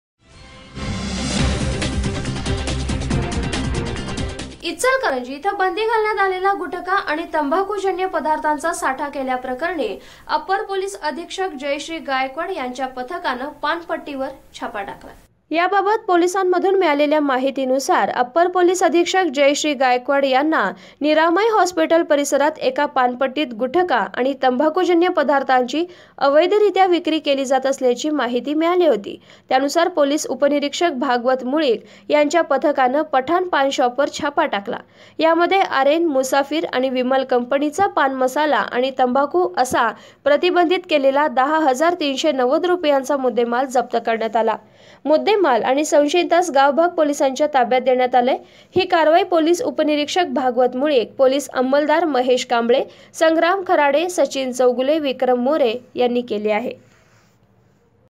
Kolhapur | इचलकरंजीत पानपट्टीवर छापा : 10 हजार 390 रुपयांचा मुद्देमाल जप्त
इचलकरंजीत पानपट्टीवर छापा : 10 हजार 390 रुपयांचा मुद्देमाल जप्त.@